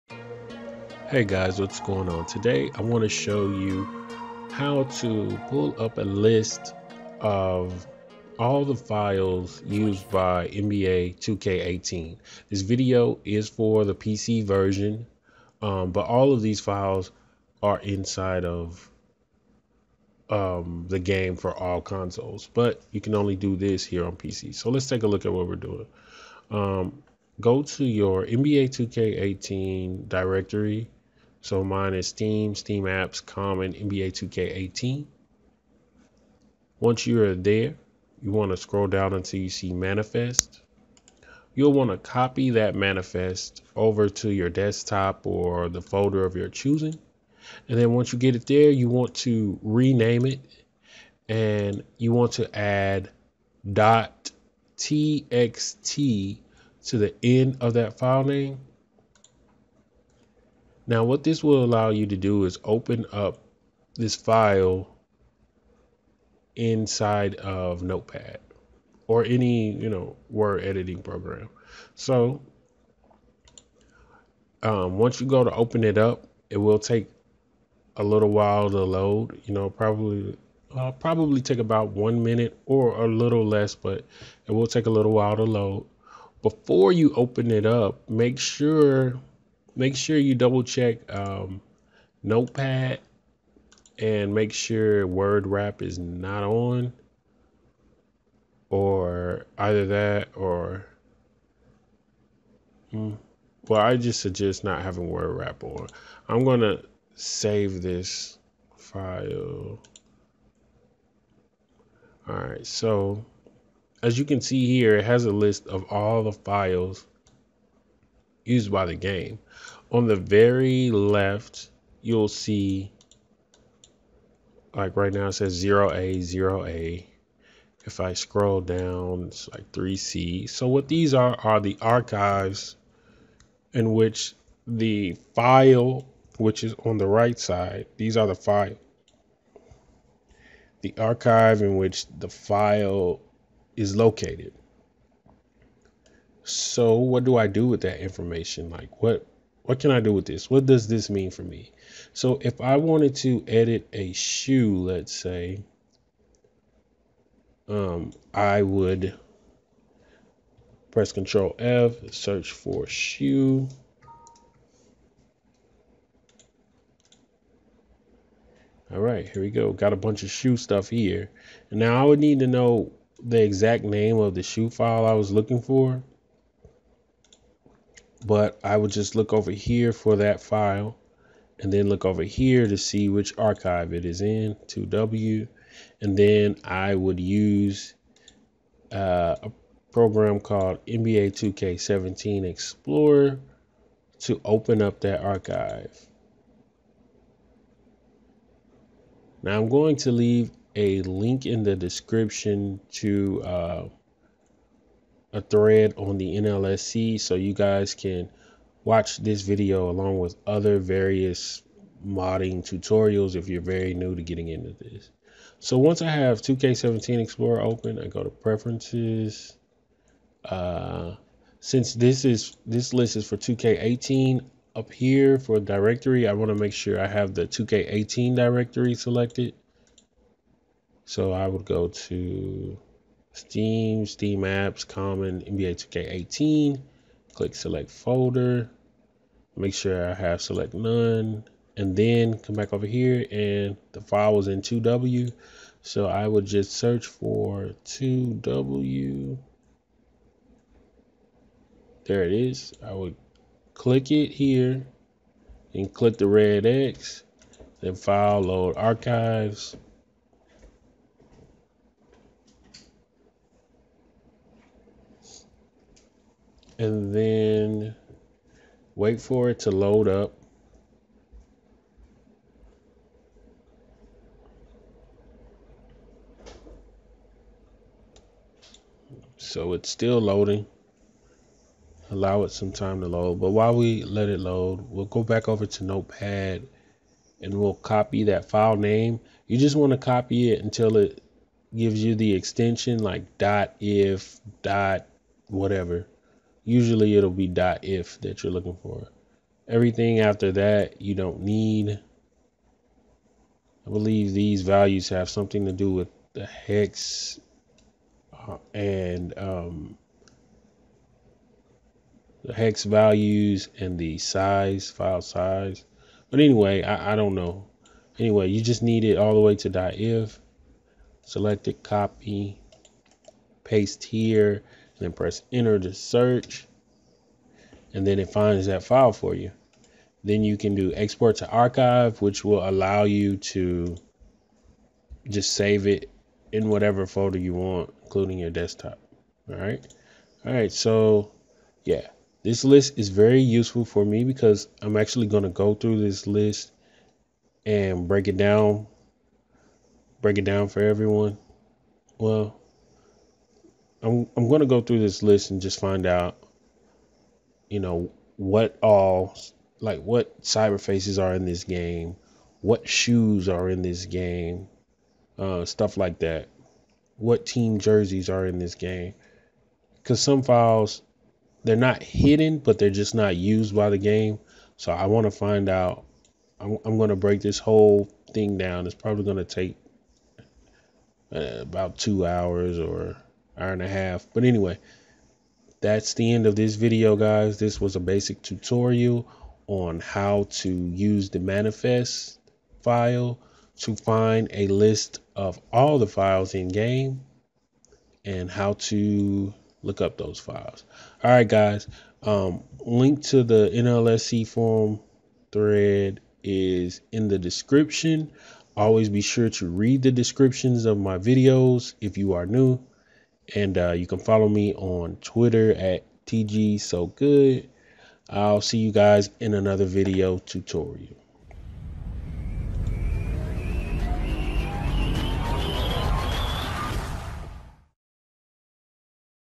How to View a list of all NBA 2K18 files ( PC Modding Tutorial ) using notepad
This video will show you how to open a list of all the game files within NBA 2K18 ( PC ) using notepad

Steam: TGsoGood
NLSC: TGsoGood
OperationSports: TGsoGood